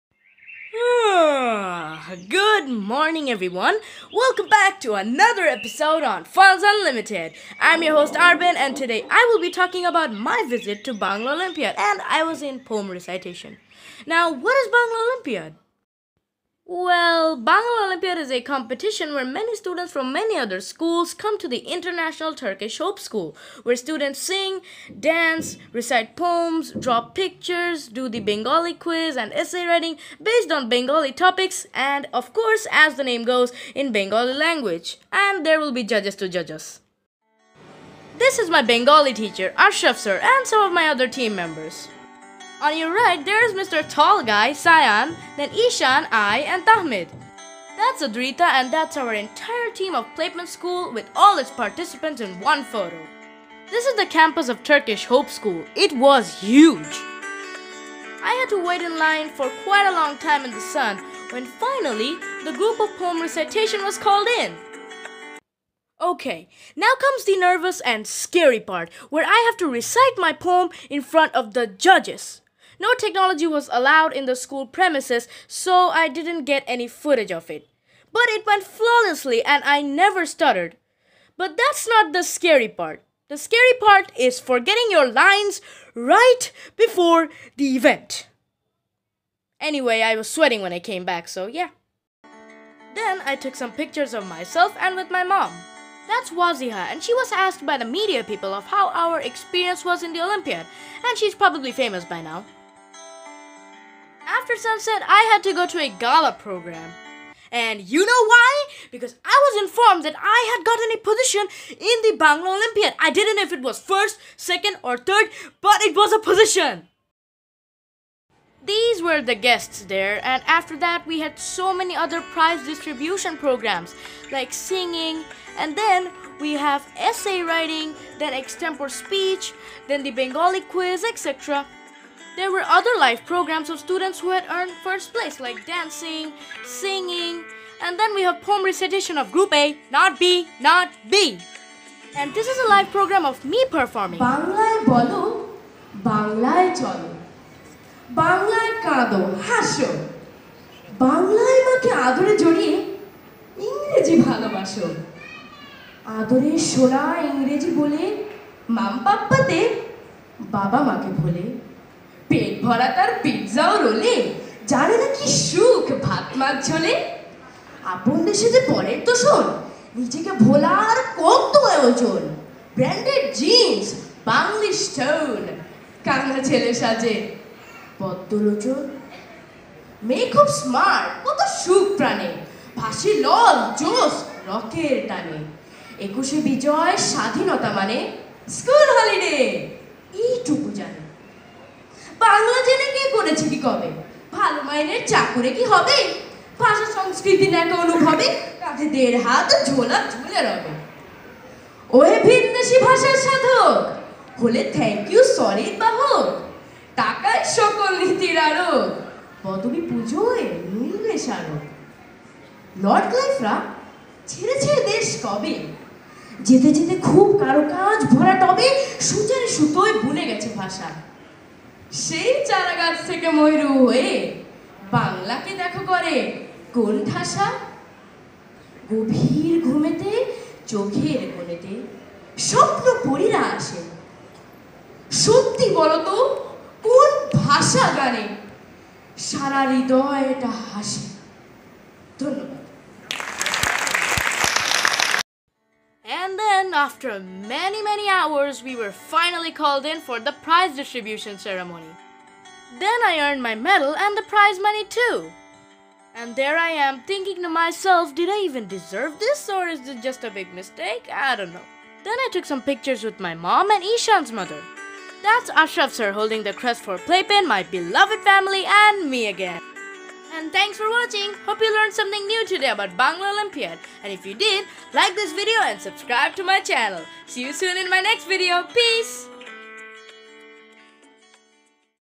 9th Interschool Bangla Olympiad !!!! 😄😊 I loved it!!!!!
Today i've made a video about my visit to Bangla Olympiad. All these pictures have been taken by my mother.LOVE YOU MOM! and soooooo. hope you enjoy the video